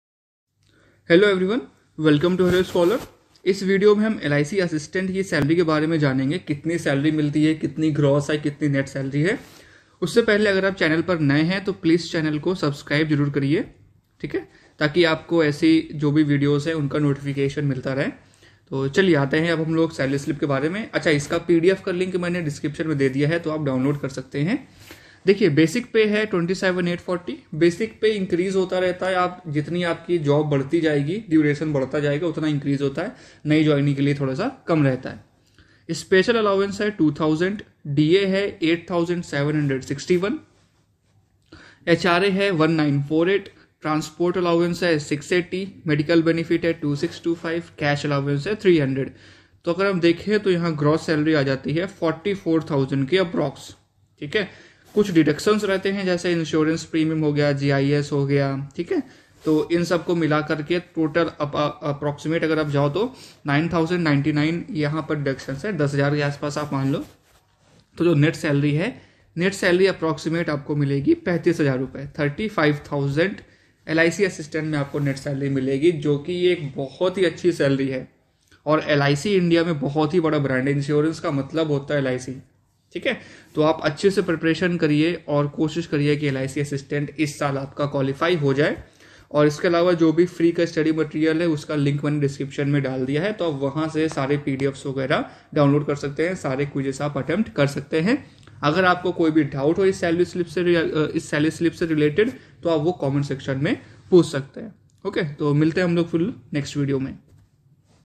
LIC Assistant Latest Salary Slip 2022 | Gross and Net in hand Salary
LIC Assistant Latest Salary Slip 2022 . let's have a look at Gross and Net in hand Salary.

LIC Assistant is a very good job. It offers a decent salary with a lot of allowances. Prepare well and qualify the exam..!!